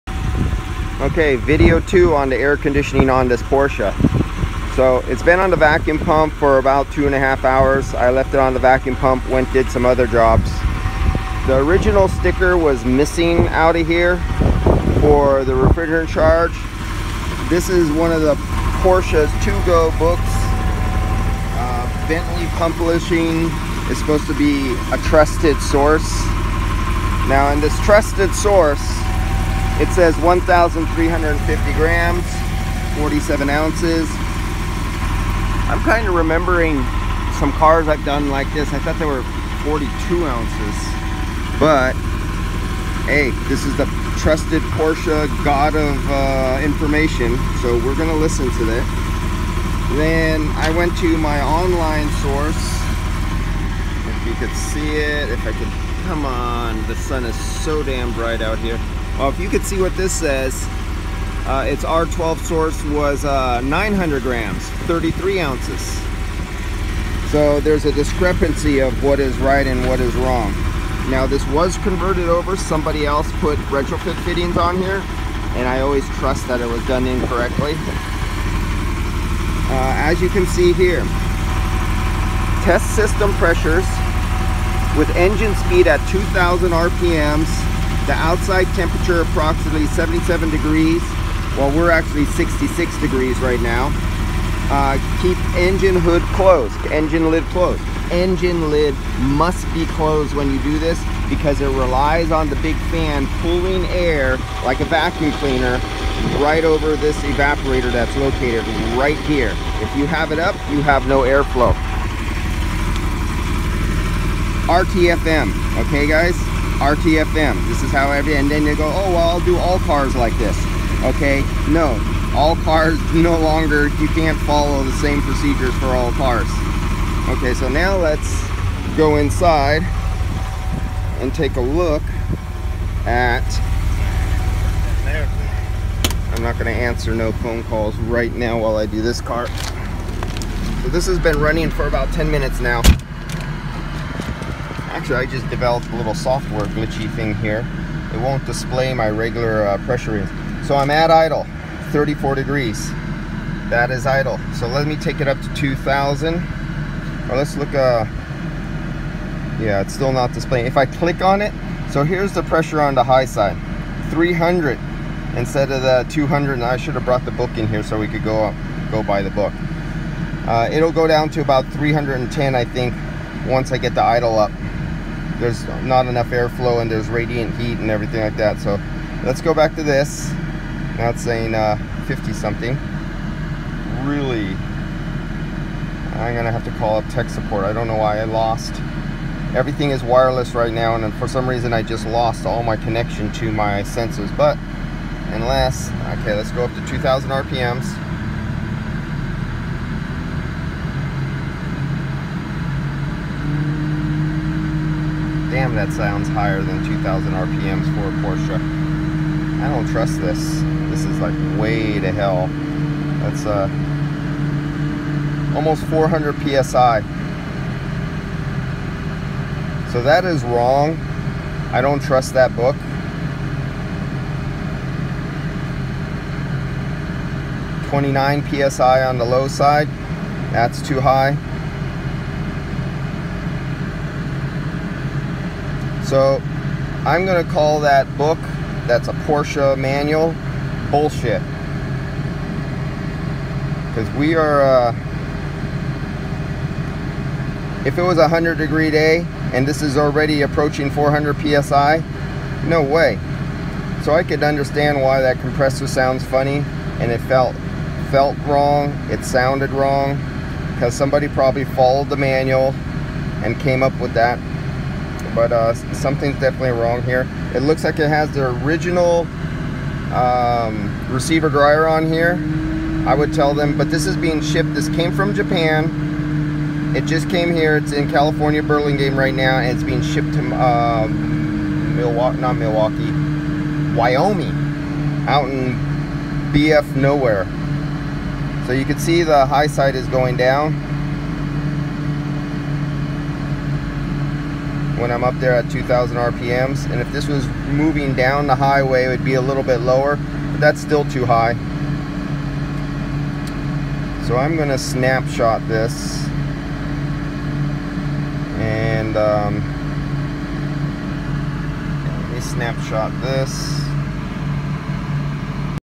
AC Pressures using Bluetooth wireless Fieldpiece￼ sensors￼. Engine compartment lid MUST be closed.￼
This Is where wireless Bluetooth gauges work perfectly
Remember This vehicle has been converted from R12 to R134￼ So it would not be filled to 100% of the R 12 refrigerant charge capacity of 1400 g it would be less .  Because R134 expands more than R12￼￼ In the type of condenser on this vehicle is not really great for doing they R130 for a retrofit on￼.